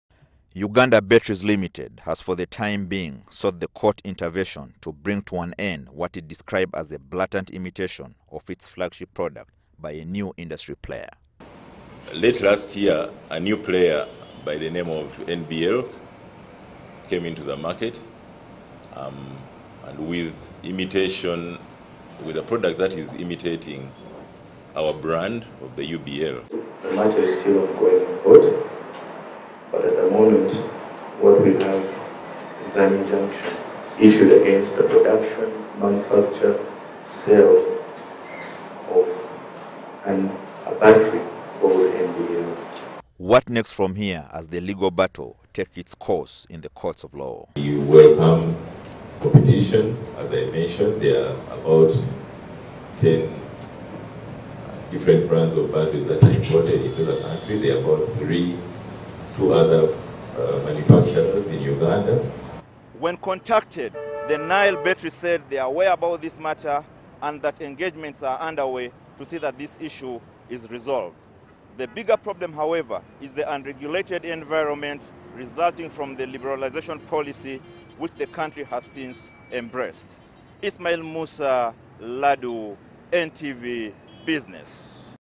Uganda batteries Limited drags rival to court over alleged imitation
Uganda Batteries has run to court to stop what they call imitation by a new investor of their Batteries’ design and logo threatening their market position.

According to a communication however issued by the Registration Services Bureau, the registration of logo and trademark by Nile Batteries Limited had been rejected